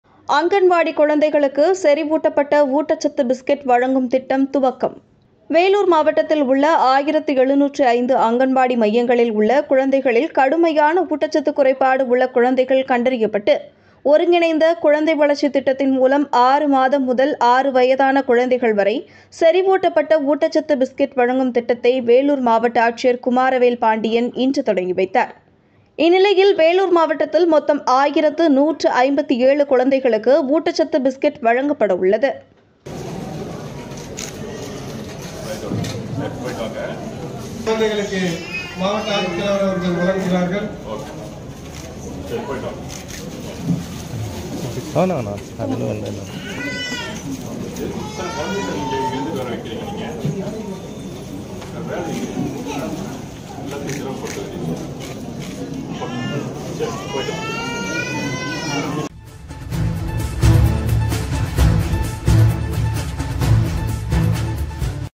அங்கன்வாடி குழந்தைகளுக்கு செறி ஊட்டப்பட்ட ஊட்டச்சத்து பிஸ்கட் வழங்கும் திட்டம் துவக்கம் | வேலூர்
அங்கன்வாடி குழந்தைகளுக்கு செறி ஊட்டப்பட்ட ஊட்டச்சத்து பிஸ்கட் வழங்கும் திட்டம் துவக்கம்